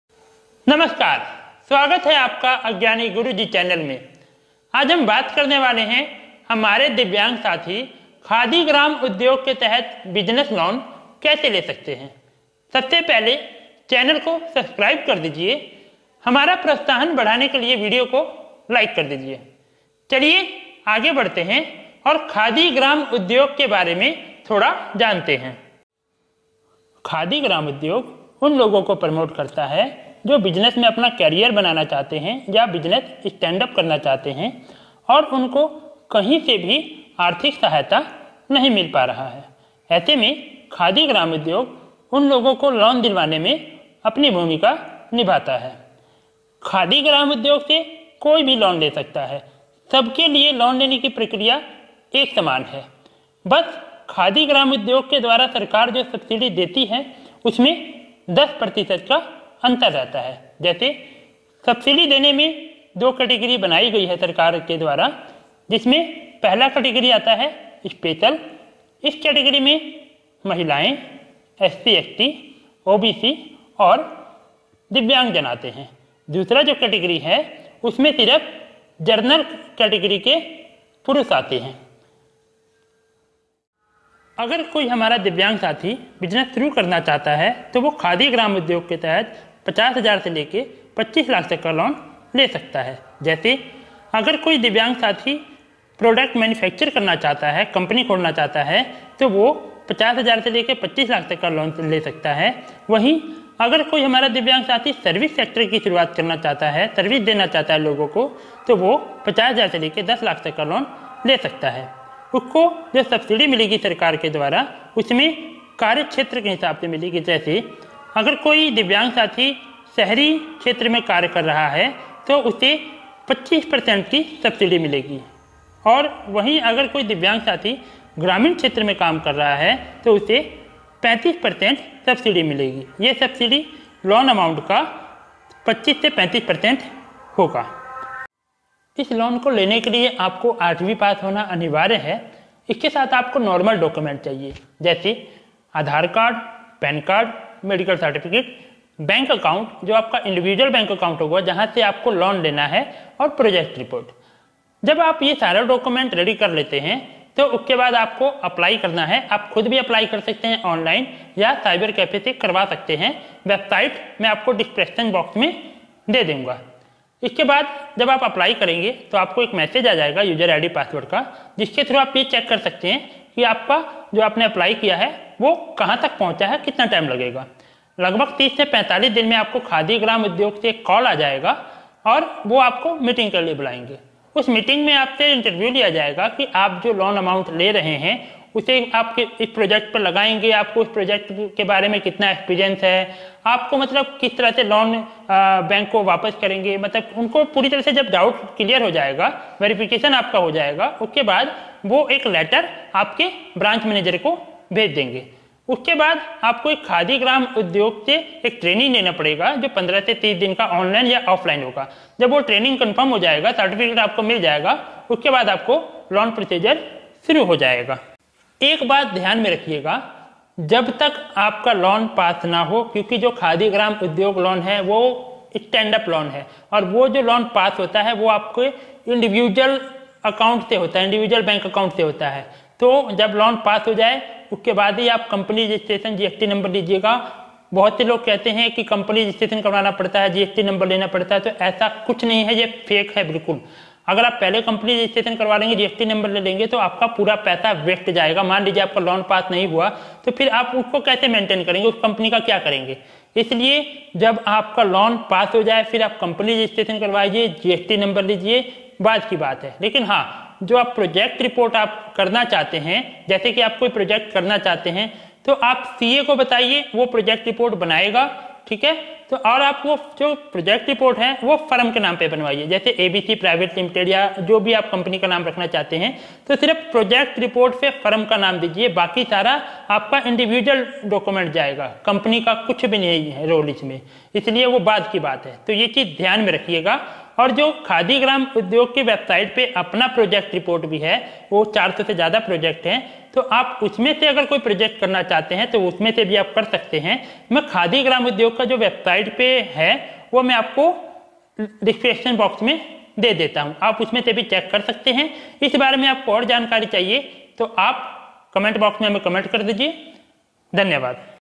खादी ग्रामोद्योग योजना (Part - 1) | Business loan | How to apply for Khadi Gram Udyog
In this video, I'm going to teach you the step-by-step process of applying for a business loan through Khadi Gram Udyog. If you're interested in starting or expanding a business, then this video is for you!

I'll cover the different types of business loans available, how to apply and how to negotiate the best terms. I hope that this video will help you apply for the business loan you need to start or expand your business!